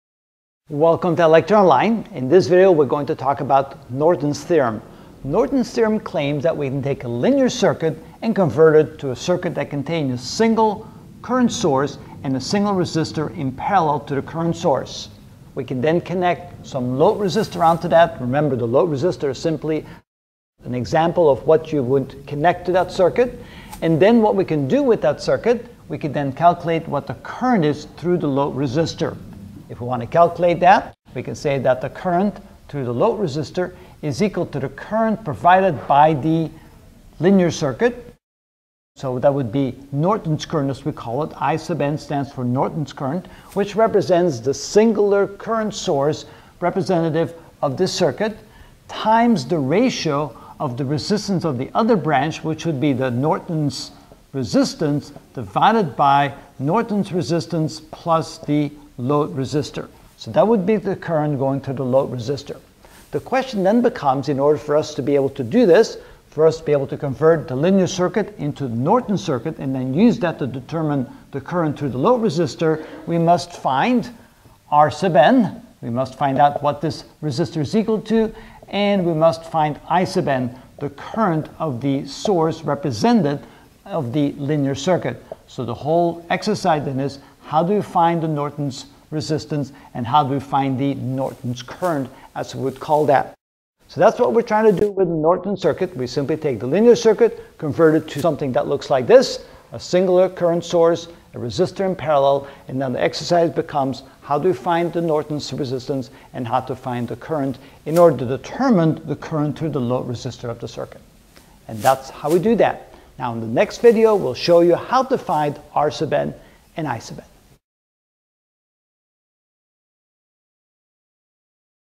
Electrical Engineering: Ch 4: Circuit Theorems (18 of 32) Norton's Theorem
In this video I will explain Norton's theorem of converting a linear circuit to a single current source and resistor in parallell.